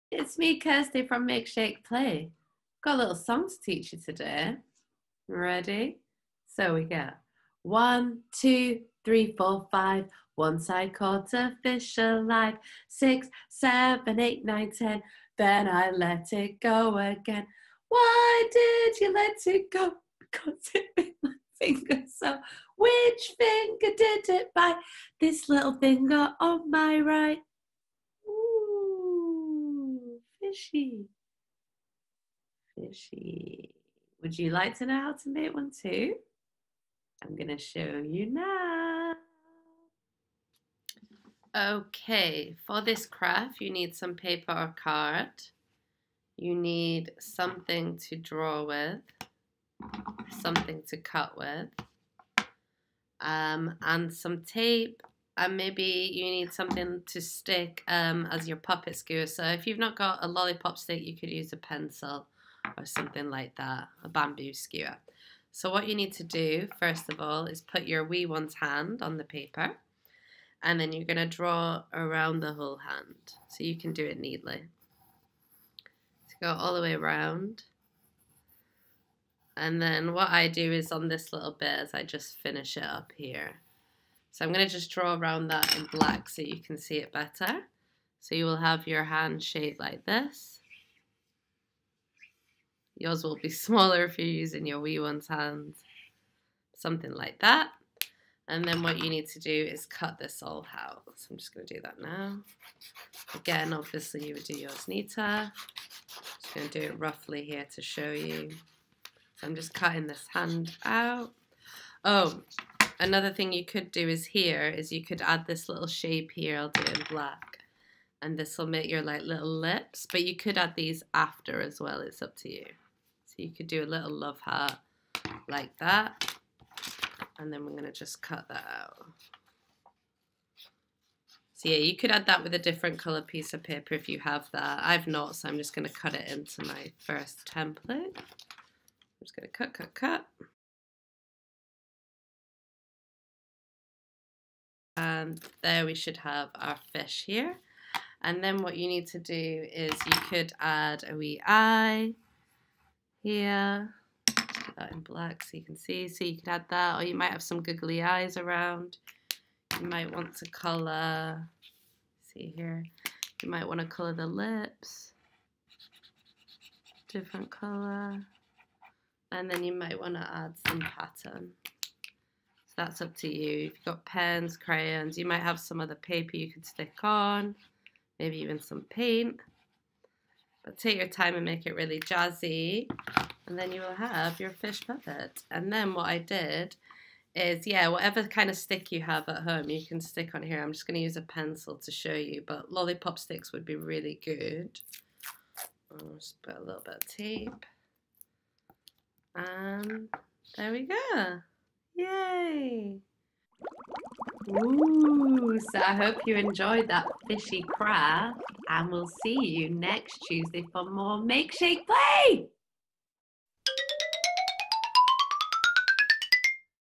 Make your own Fish Finger Puppets | Make Shake Play
Bet you've never seen fish fingers like these before. Join Kirsty from Mildmay Community Centre and learn how to make these fun underwater friends.

You will need: paper, pens, colouring in pencils or felt tips, a stick, tape, a pair of scissors and a responsible adult to help you cut out your fish puppet.